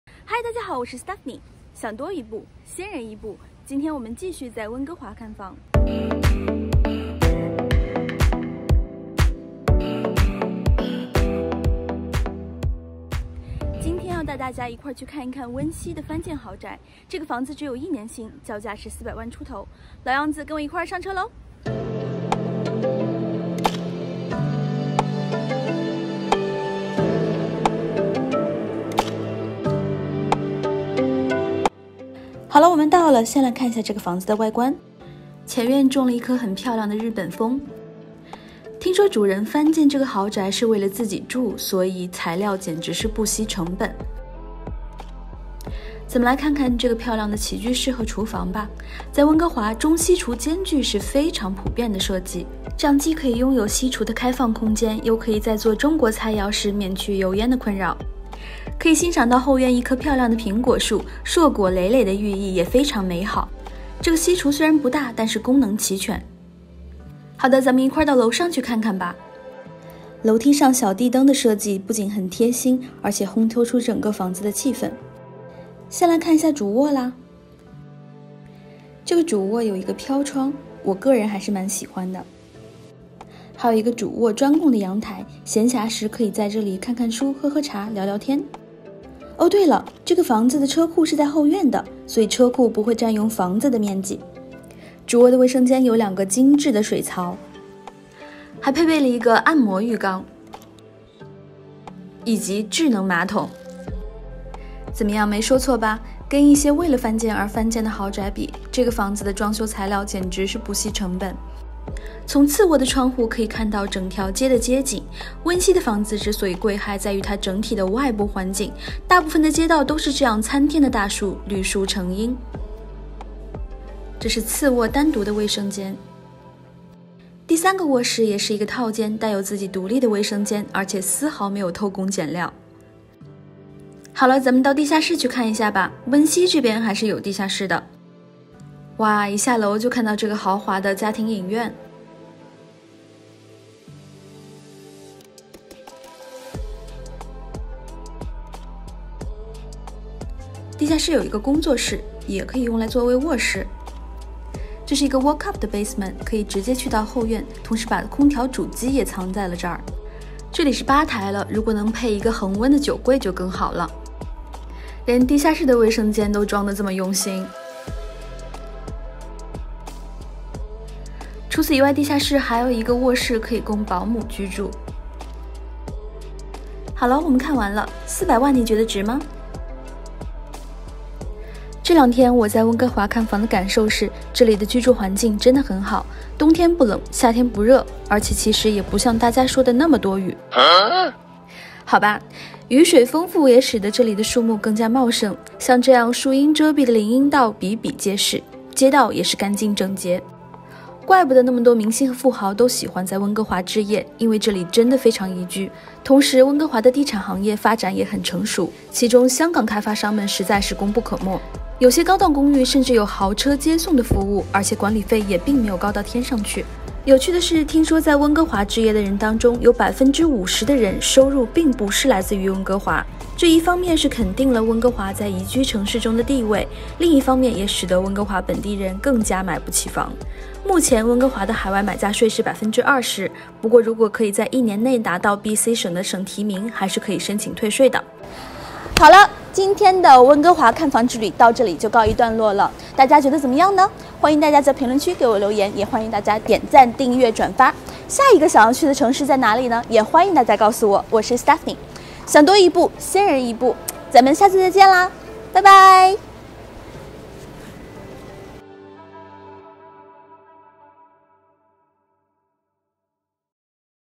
【 看房VLOG#11】为什么明星喜欢在温哥华买房？多伦多地产经纪带你去温哥华看房|400余万温西翻建豪宅
在温哥华看房的感受是，这里的居住环境真的很好。冬天不冷，夏天不热。而且其实也不像大家说的那么多雨。好吧，雨水丰富也使得这里的树木更茂盛。像这样树荫遮蔽的林荫道比比皆是。街道也是干净整洁。怪不得那么多明星和富豪都喜欢在温哥华置业，因为这里真的非常宜居。同时温哥华的地产行业发展也很成熟，其中香港开发商们实在是功不可没。有些高档公寓甚至有豪车接送的服务，而且管理费也并没有高到天上去。有趣的是，听说在温哥华置业的人中有50%的人，收入并不是来自于温哥华。这一方面是肯定了温哥华在宜居城市中的地位，但另一方面也使得温哥华本地人更加买不起房。目前温哥华的海外买家税是20%，不过如果可以在一年内拿到BC省提名还是可以申请退税的。

此次视频特别感谢温哥华地产经纪Charlie和Tiffany，如有需要欢迎与他们联系：